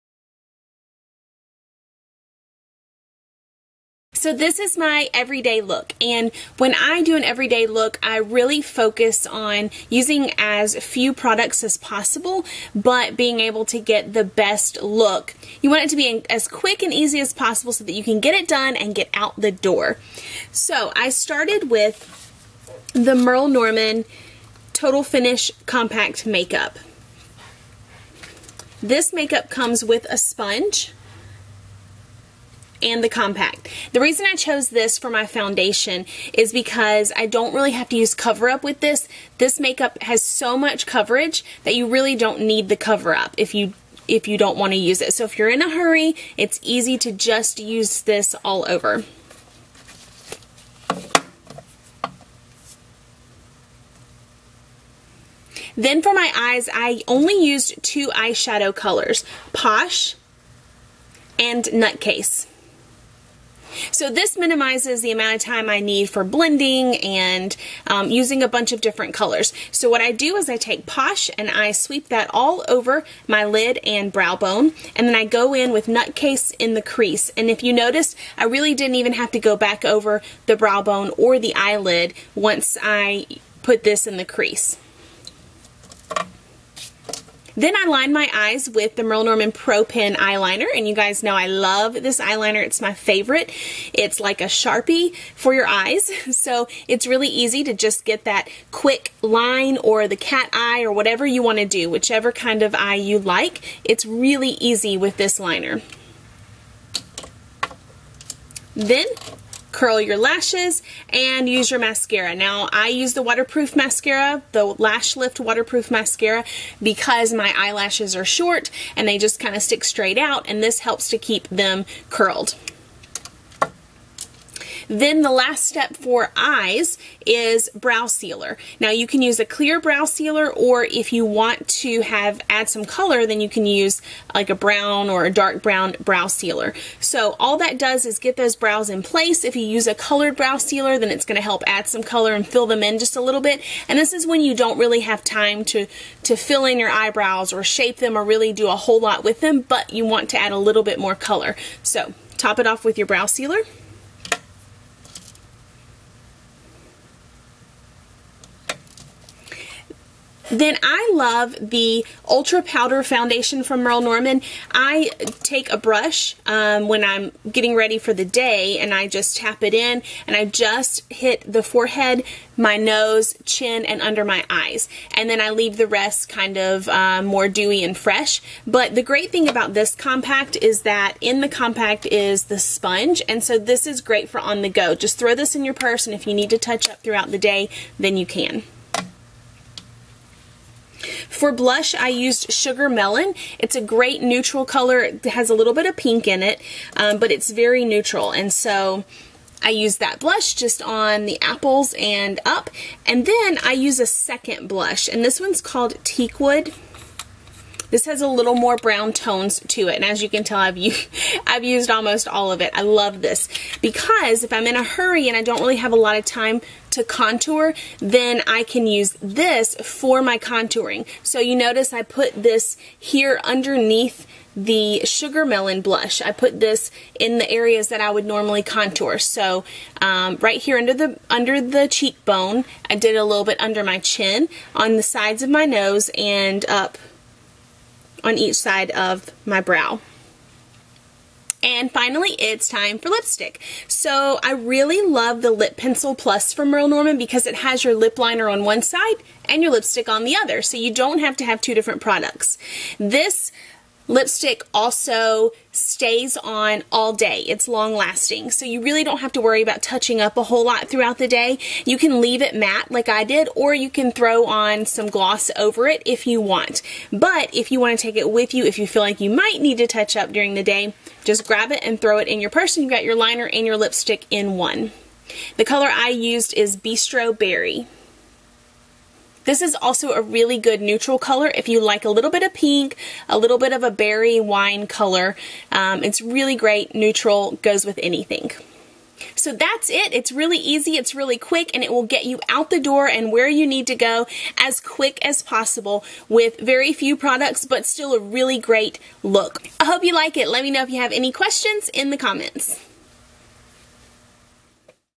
CHARISSA'S EVERYDAY TUTORIAL {MERLE GIRLS}
Products used:

Total Finish Foundation
Post Eyeshadow
Nutcase Eyeshadow
Pro Pen Eyeliner in Sharp Black
Lash Lift Waterproof Mascara in Black
Clear Brow Sealer
Ultra Powder Foundation in Chiffon
Blush in Sugar Melon
Blush in Teakwood (for light contouring)
Lip Pencil Plus in Bistro Berry
* All products used were from Merle Norman Cosmetics